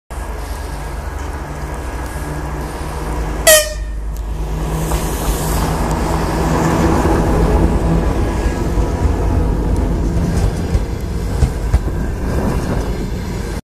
Тепловоз ТЭП70БС-232 с пригородным поездом Ижевск - Сайгатка
Тепловоз ТЭП70БС-232 с пригородным поездом Ижевск - Сайгатка отправляется со станции Ижевск, г. Ижевск, Удмуртия, Россия. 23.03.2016